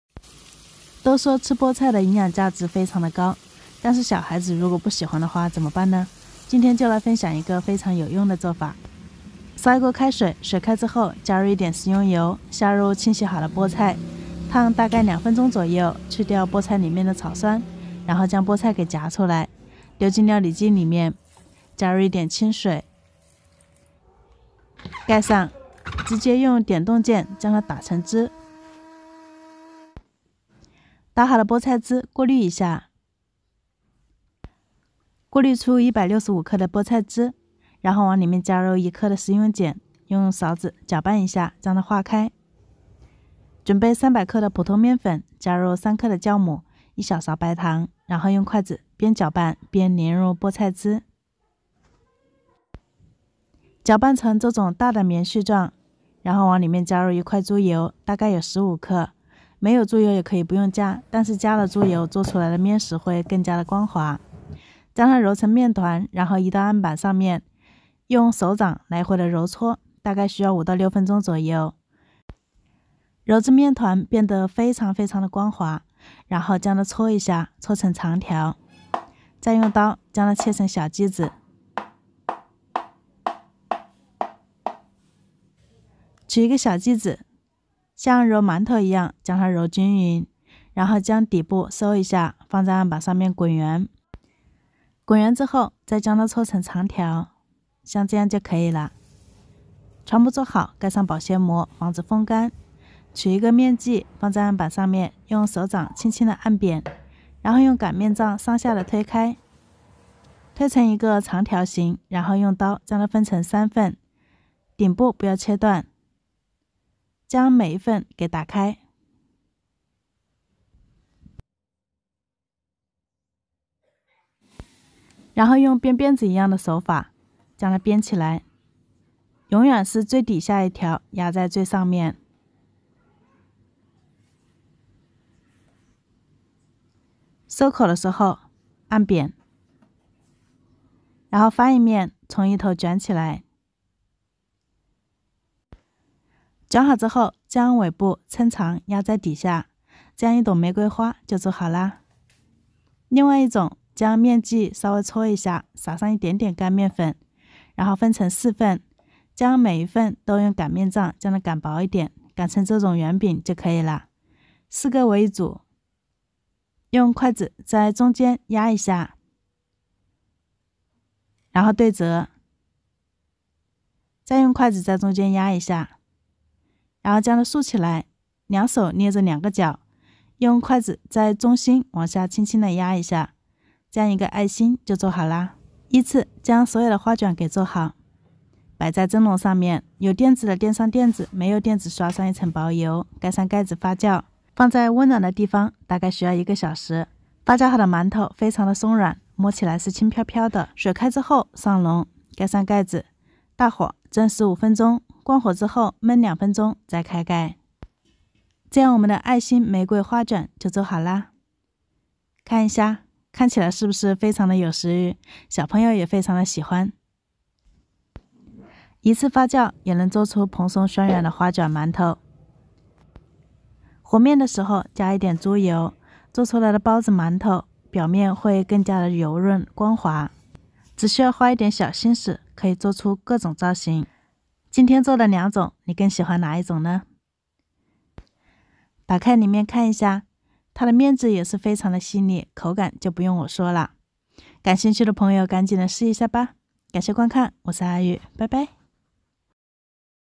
花卷好吃，和面比例是關鍵，技巧告訴你，不用二次發面，宣軟蓬松
每天都會為你更新一道中國#美食 家常菜 面食 小吃 的做法

阿玉愛美食
我是中國湖北人，謝謝您的喜歡